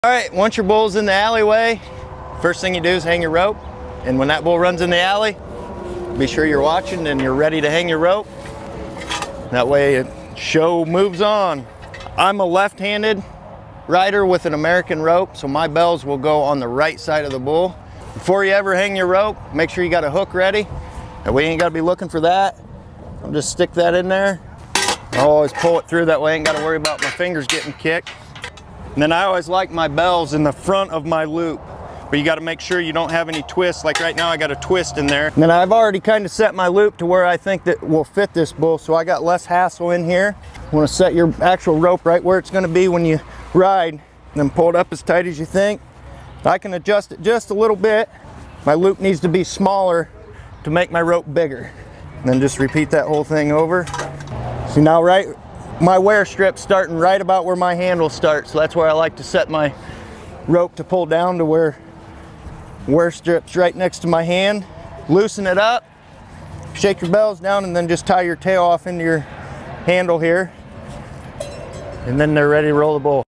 How To Hang Your Bull Rope
When your bull runs in the alley here's how to hang that rope.